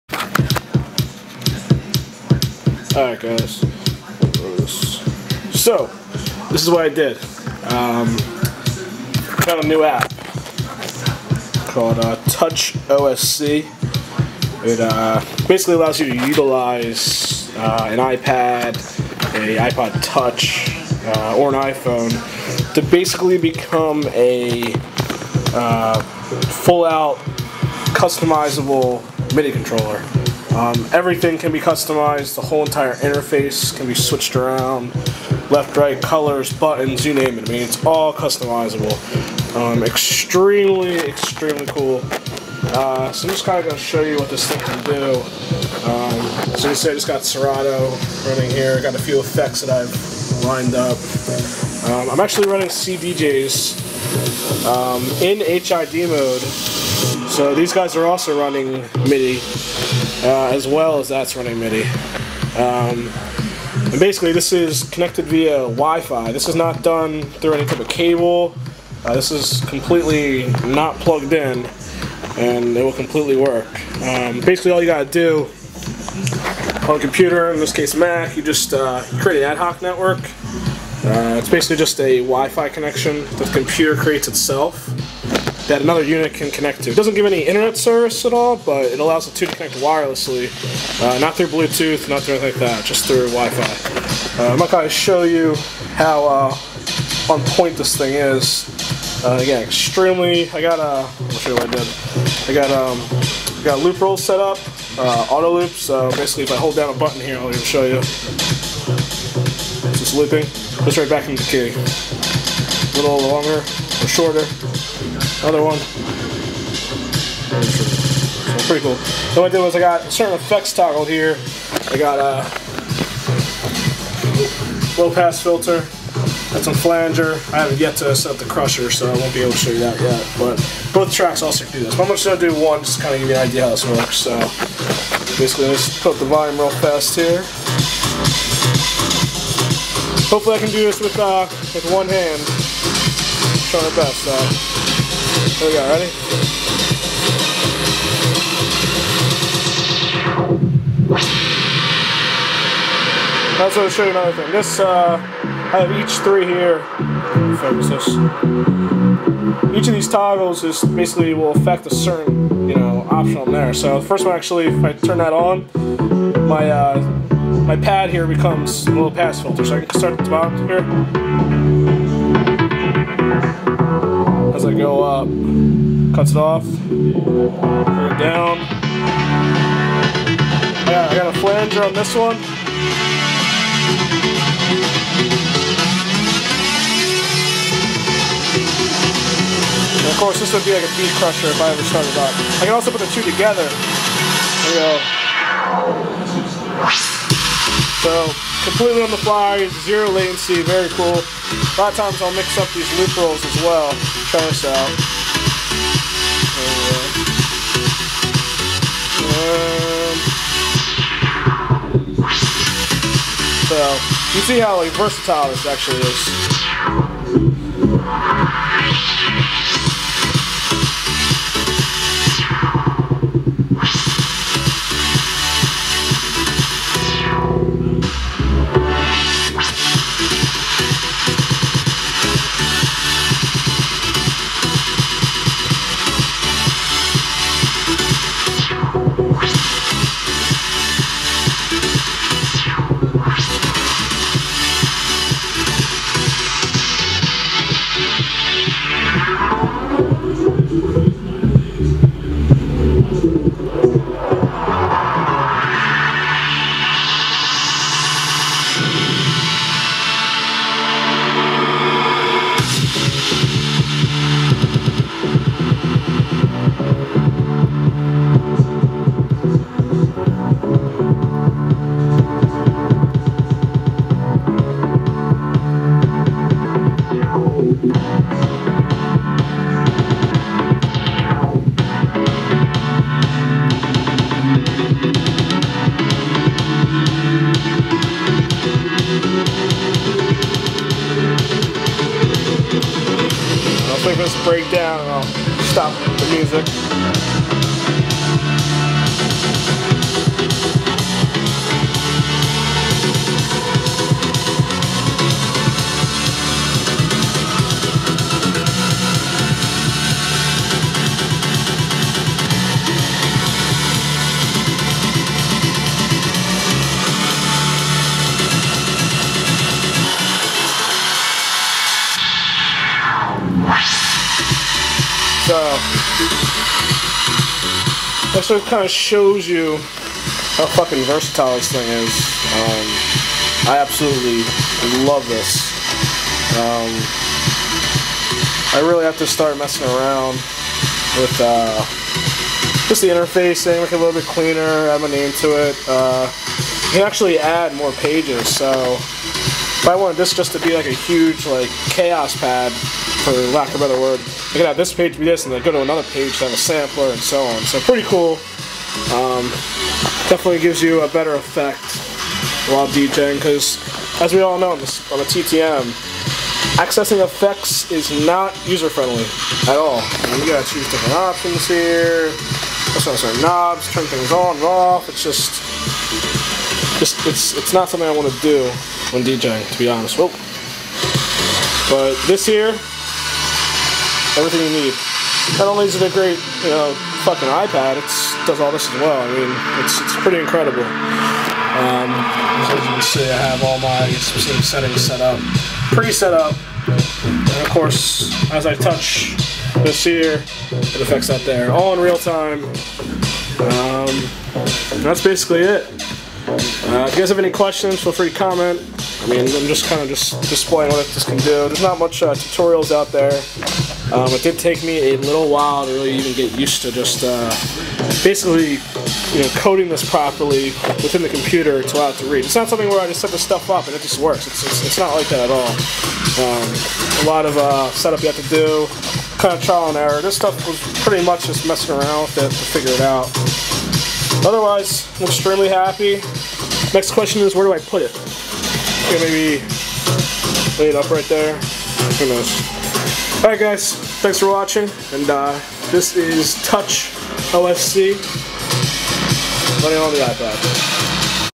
TouchOSC on iPad with Serato
So I've been trying to find a cool way to have a midi controller accessible to me while DJing. I really like to cut up my music and add effects on the fly; thank god I finally found the best way to do just that

This is touchOSC running on my iPad. Its a fully customizable interface that basically interacts with any program that runs MIDI. In this case I was running it through Serato.

Extremely flexible. Completely customizable. 100% worth it.

Here are a few links to download the essentials to set up TouchOSC with your rig.

1. Download an application called 'TouchOSC' on your iOS or android device. ($4.99)

2.

3. Download an application called Osculator on your computer

This is the app that converts the signal sent by TouchOSC (Open Sound Control) into a MIDI signal. Keep in mind that Osculator is shareware, but unless you donate the minimum of $19 (I suggest the full $39), you will get a pop-up message every few minutes asking you to donate and when this happens, there is no signal being sent to Scratch LIve. So...make sure you donate at least $19 and you will get an activation code.

Both programs do have readme files for some help, but otherwise if you have any questions, please post in the comments section below so the answers will be visible to anyone else with the same questions.